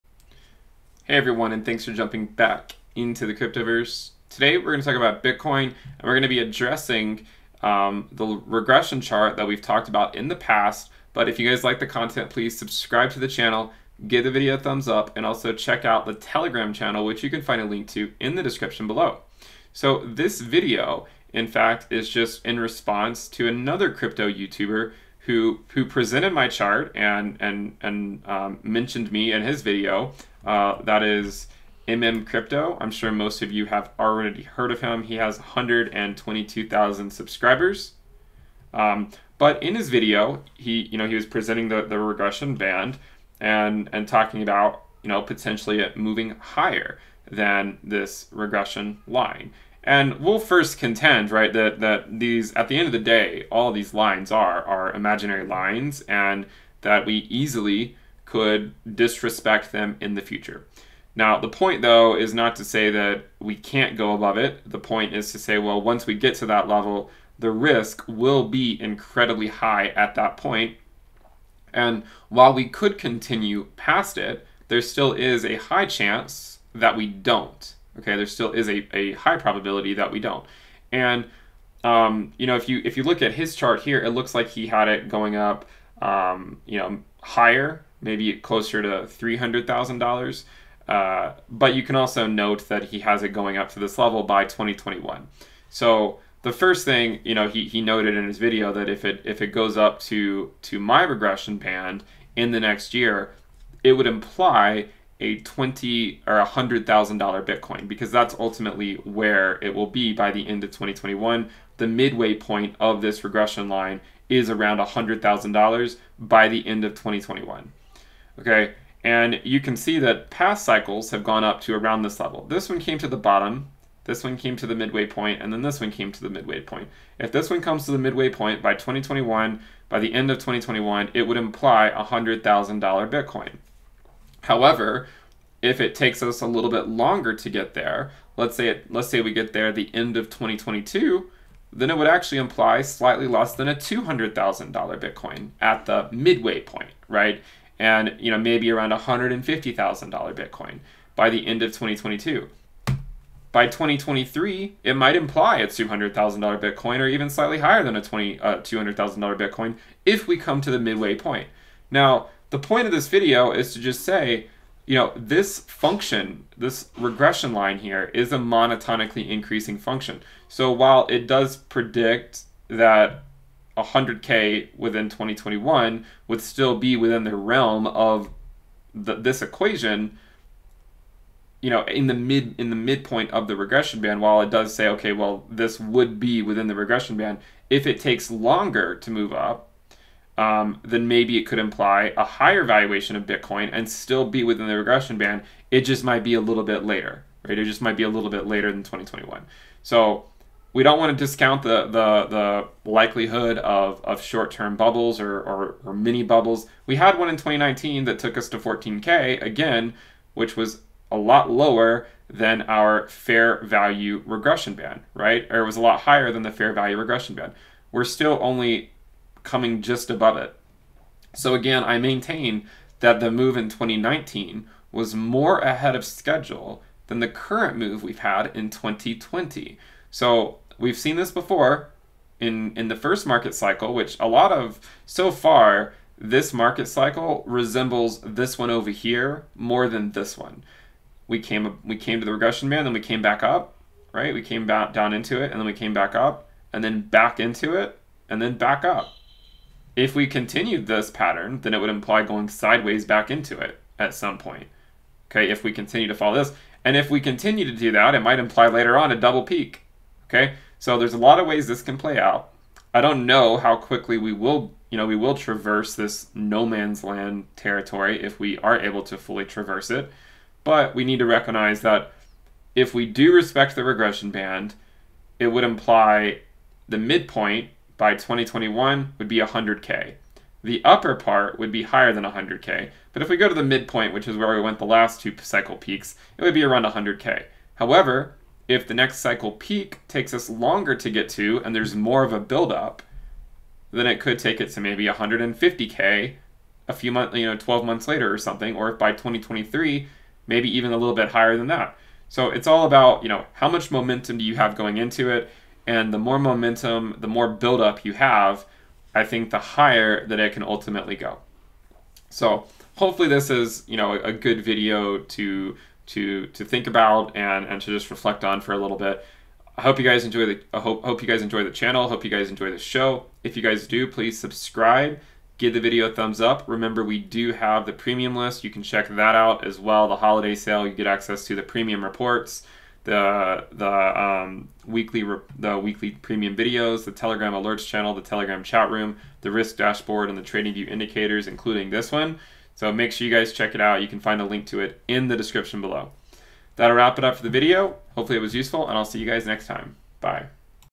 Bitcoin: Implied logarithmic regression band tops depending on year
Now that we have convincingly left the "fair-value" logarithmic band in the short-term, let us take a look higher at the peak logarithmic regression band. We identify where this regression band predicts the global top to be (based on the midway point of the regression band), based on which year we can it there. The longer it takes for Bitcoin to reach the upper regression band, the higher it should theoretically go considering the logarithmic regression band of Bitcoin is a monotonically increasing function. What year do you think we will reach the upper Bitcoin logarithmic regression band?